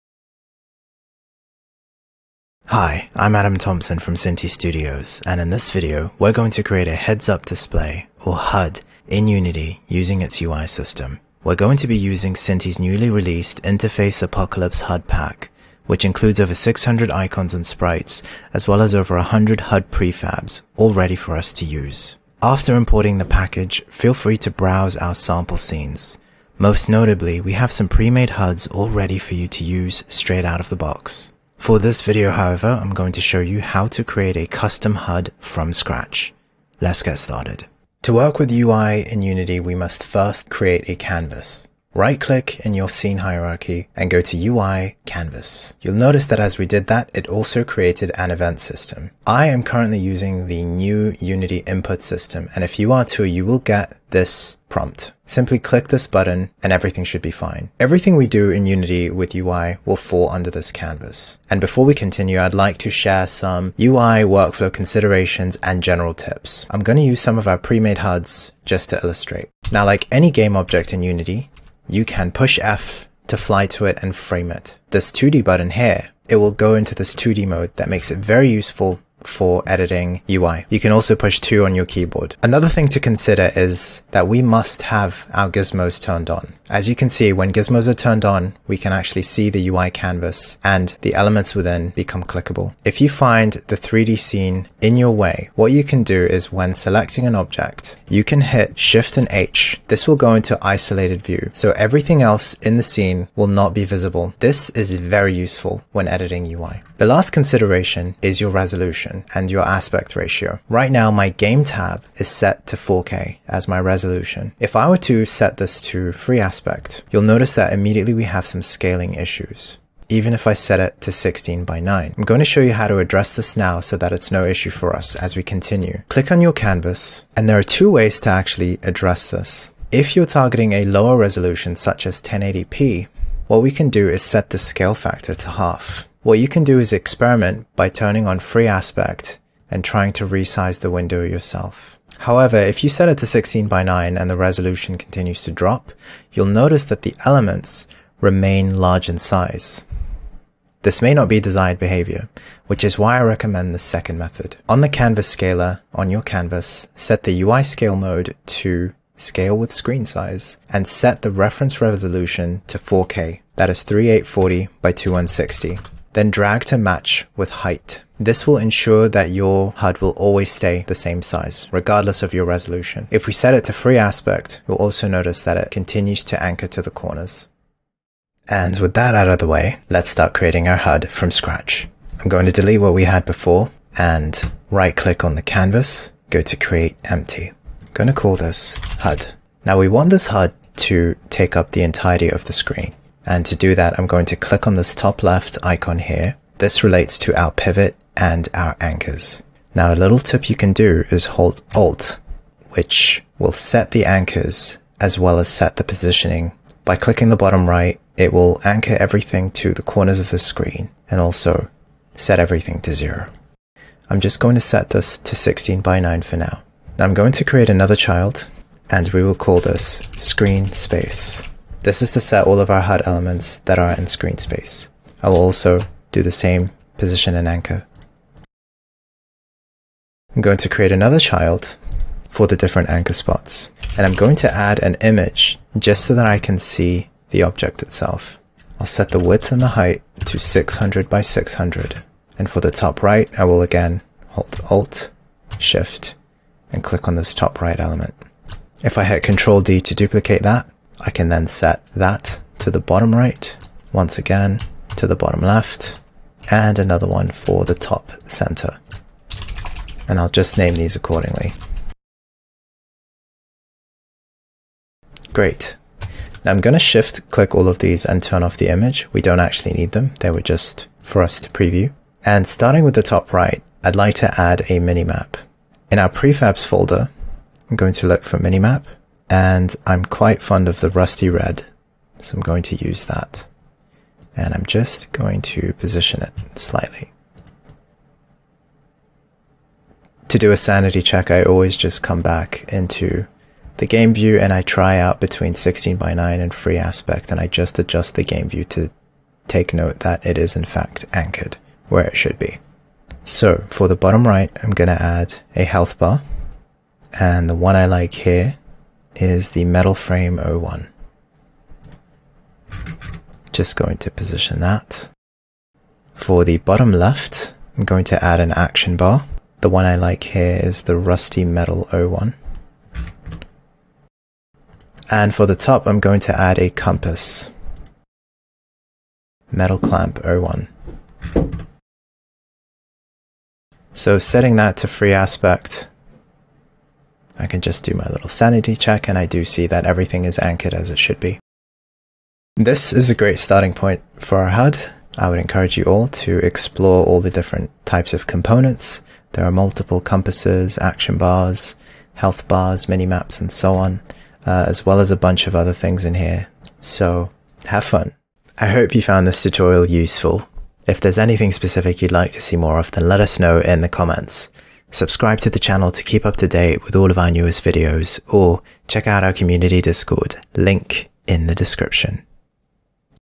Creating a HUD in Unity using the Interface Apocalypse HUD pack by syntystudios
In this video we cover how to set up your own UI HUD in Unity using the Interface Apocalypse HUD pack available now on the Synty store. We also cover some basic tips and tricks, UI scaling and how to anchor your UI to get the most out of your HUD.

* Available now for Unity - Purchase on the Synty store to access a .unitypackage

0:00 Introduction
0:37 Creating a canvas
1:01 General UI tips and tricks
1:47 UI scaling
2:58 Start creating your HUD
3:13 Anchor presets
3:36 Adding to the HUD
5:57 Outro

Check SyntyStudios for more videos of our 3D Low Poly Art for Games!